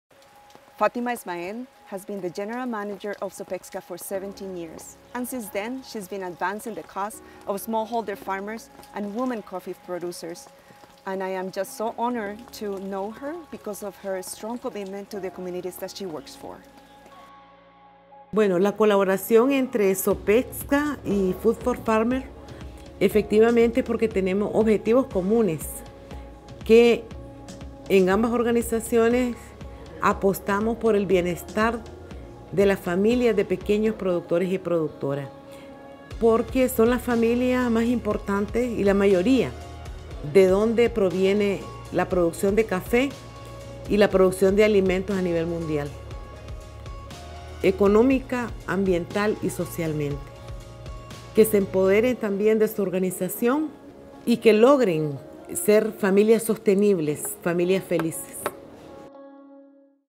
Food 4 Farmers | Fatima Ismael, Director of Sopexcca
Meet Fatima Ismael, Director of SOPPEXCCA a coffee cooperative of 520 member-families in Jinotega and Matagalpa, Nicaragua. Most families are facing up to three and a half months of food insecurity — every year. Since 2011 @Food4Farmers has been working with SOPPEXCCA on developing and implementing long-term strategies to reduce seasonal hunger and help families build a more secure and diverse economic foundation, right on their farms. Philz Coffee is a proud supporter of Food 4 Famers and SOPPEXCCA.